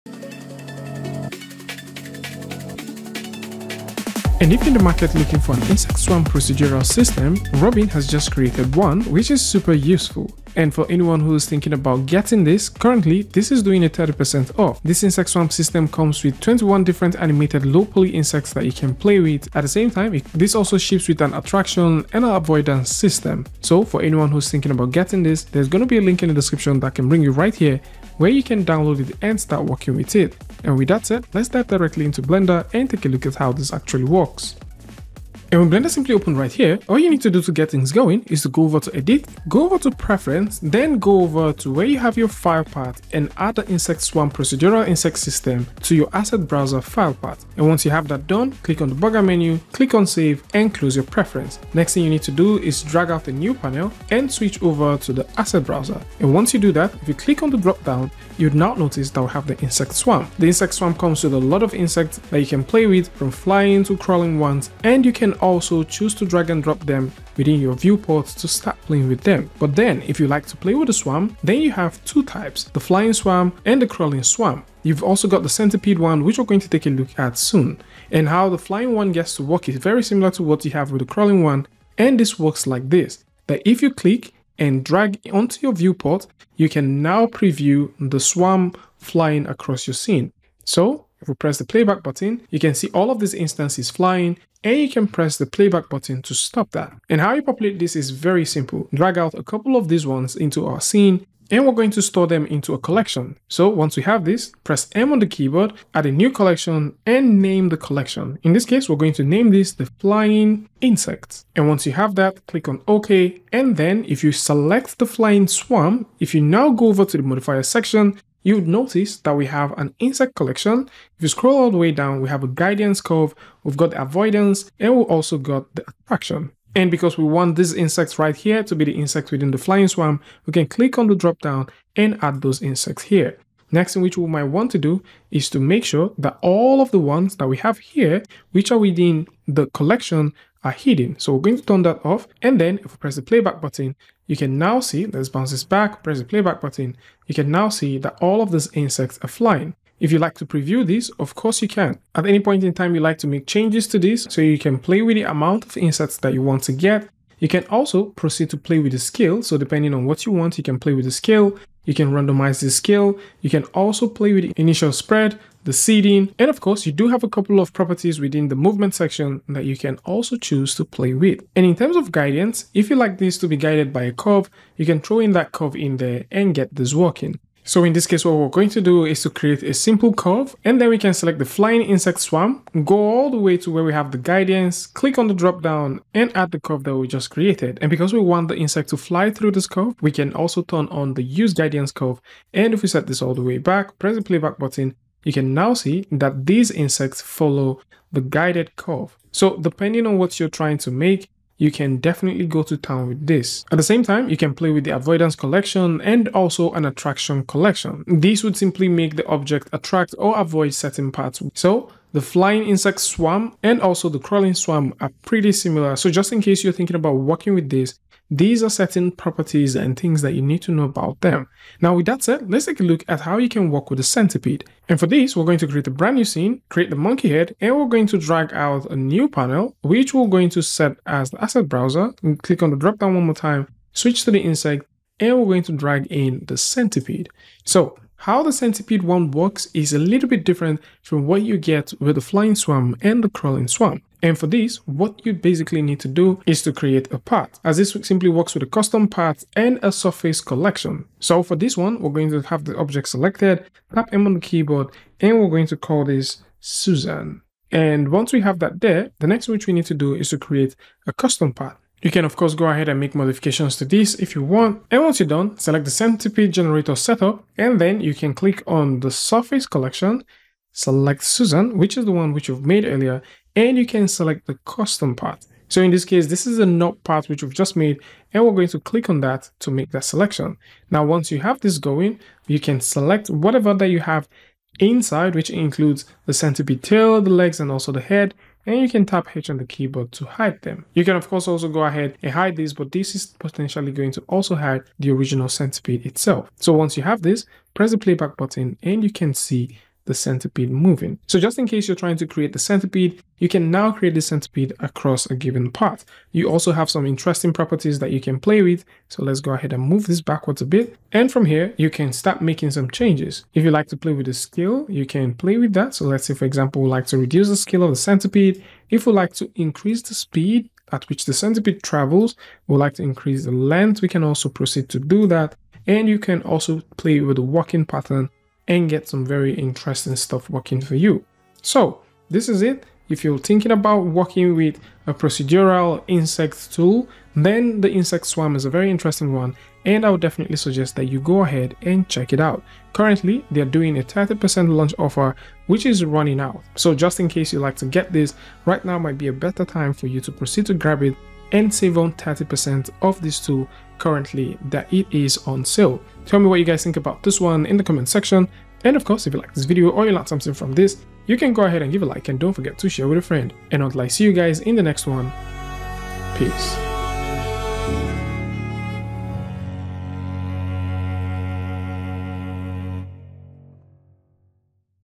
Swarm - New Procedural Insect System!
If you are looking for a procedural system for making flying and crawling insects, look no further.

00:00 Intro
00:35 Installation
01:00 Insects
01:22 Procedural Systems
02:34 Exploring Insect Systems
02:56 Using Paths
03:46 Centipede
05:44 Final Words

**Artworks us ed on thumbnails are to support artists.